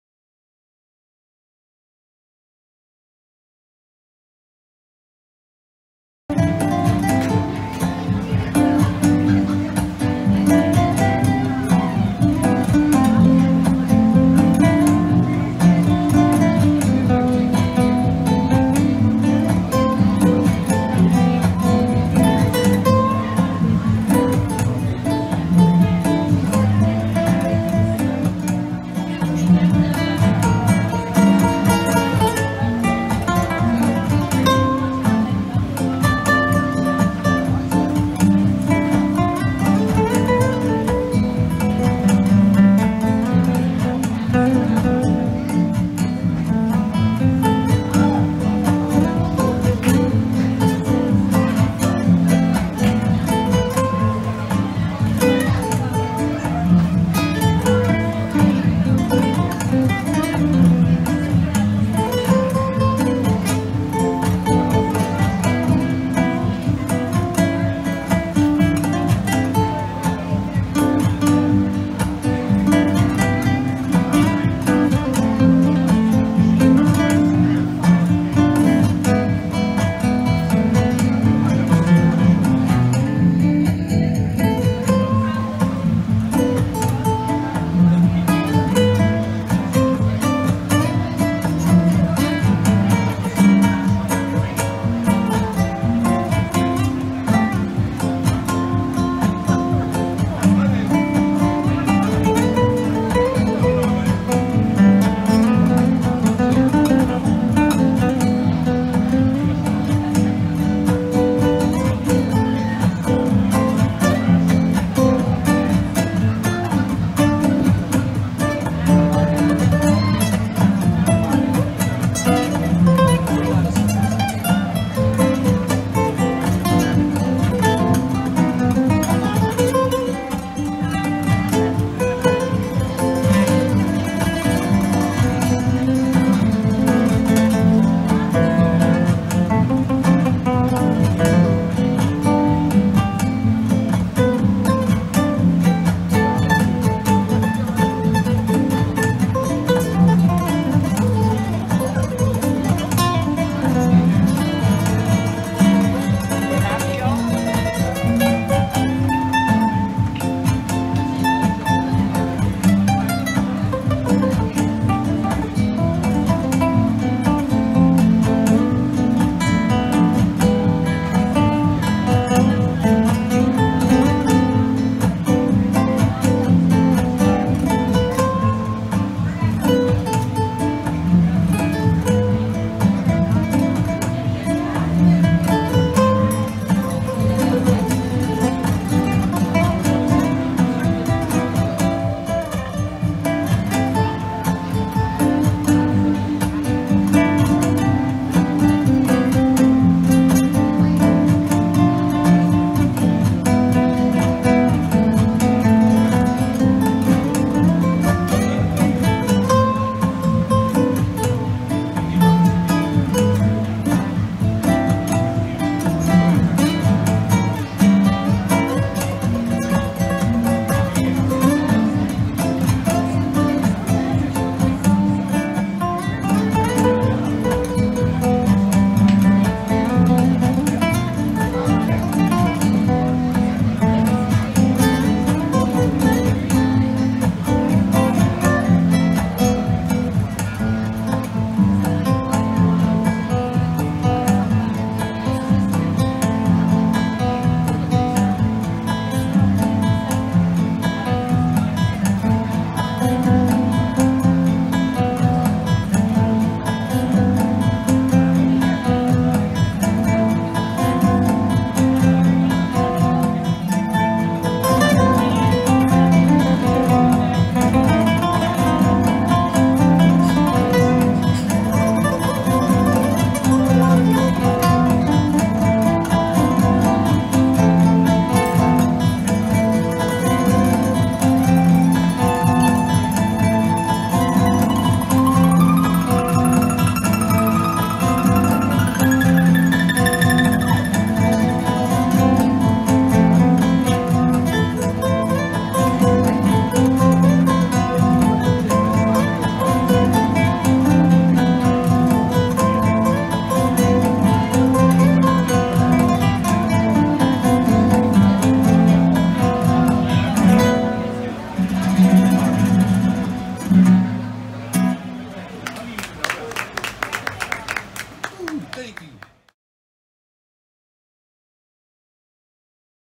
JAYAR MACK presents...BRYAN SPAINHOWER!
Anytime you need warm, calm, soothing music that puts the soul at rest and the mind at ease, please come take a listen to a good friend of mine who's extremely gifted, talented and fully well versed in the ways of the guitar...Ladies and Gentlemen, may I proudly introduce...Mr. BRYAN SPAINHOWER!!! - JAYAR MACK

Video Shot, Edited & Directed by ROBIN STONE & BARBARA JEAN BAKER for JACODA RECORDS & ENTERTAINMENT, INC.

Live at The Cottage, Siesta Key, Sarasota, FL.